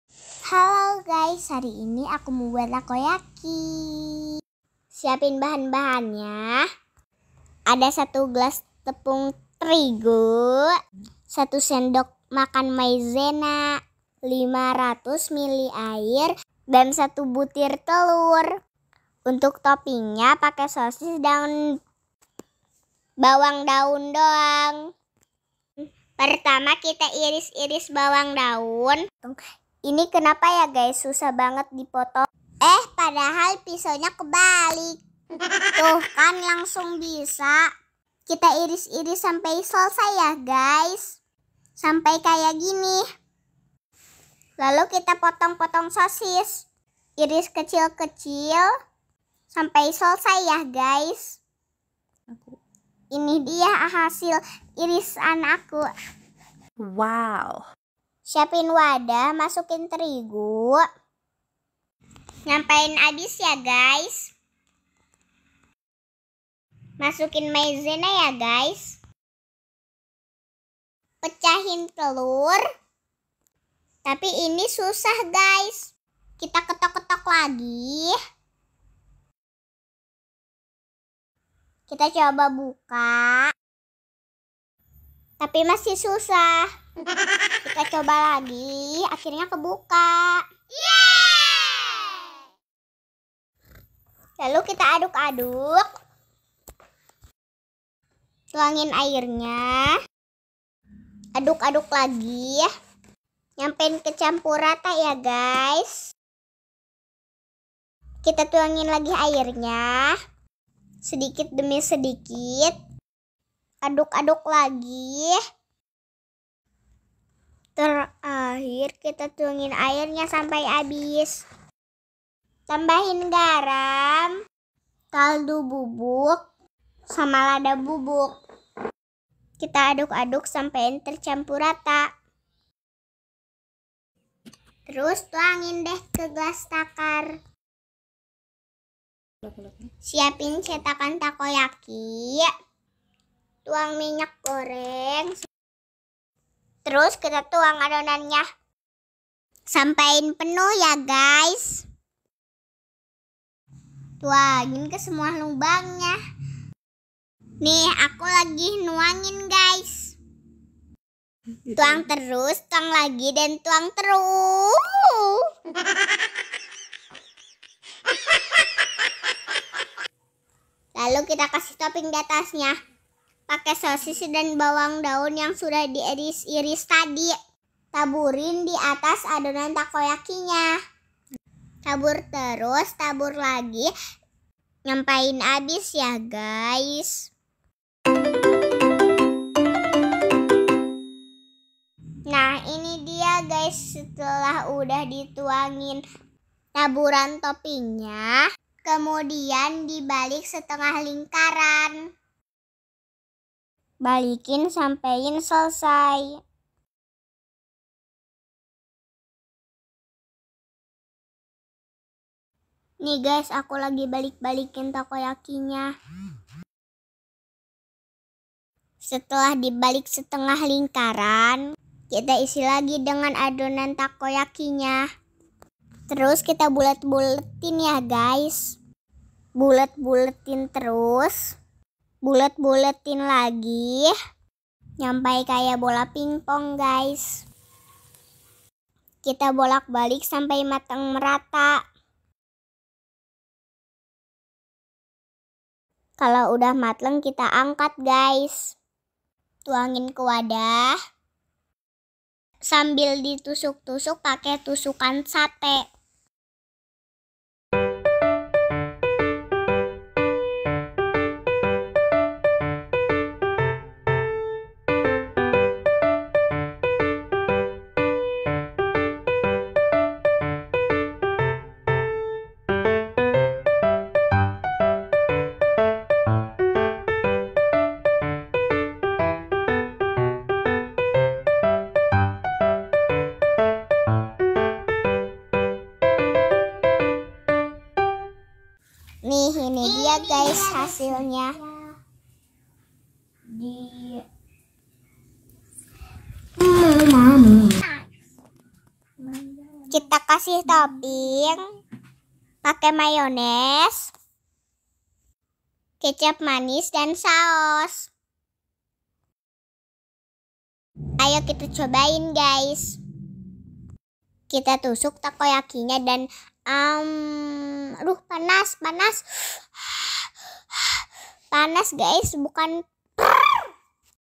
resep takoyaki simpel bahan mudah dan murah
halo guys kali ini yang masak bocil mamoy yaa...
kita bikin takoyaki kesukaan nya...
bahan seadanya menu mewah , resep mudah ,harga murah..
bahan adonan takoyaki :
* 1 gelas tepung terigu
* 1 sdm maizena
* 1 butir telur
* 500ml air
* 1/2 sdt garam
* 1/2 bks kaldu bubuk
* secukupnya lada bubuk
Toping :
sosis
bawang daun